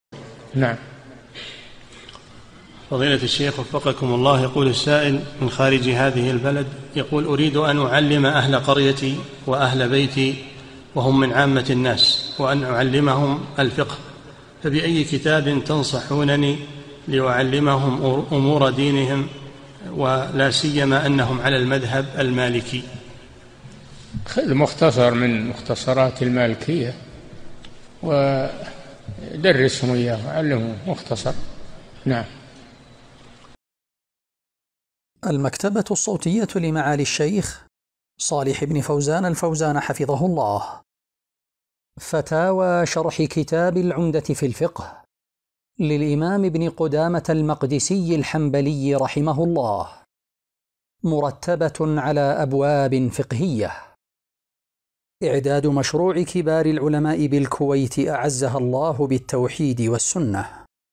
[184 -3022] أريد أن أعلم أهل قريتي الفقه، فبأي كتاب تنصحونني لأعلمهم ولاسيما على المذهب المالكي؟
-"[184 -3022] السؤال: السائل من خارج هذه البلد يقول أريد أن أُعلِّمَ أهل قريتي وأهل بيتي وهم من عامة الناس وأن أعلمهم الفقه، فبأي كتابٍ تنصحونني لأعلمهم أمور دينهم ولاسيما على المذهب المالكي؟
الجَوَابُ: خذ مختصر من مختصرات المالكية، ودرسهم إياه وعلمهم مختصر، نعم.
للاشتراك في قناة الشيخ صالح الفوزان

للاستفسار والإقتراحات والدعم
رئيس الفريق
لمشروع كبار العلماء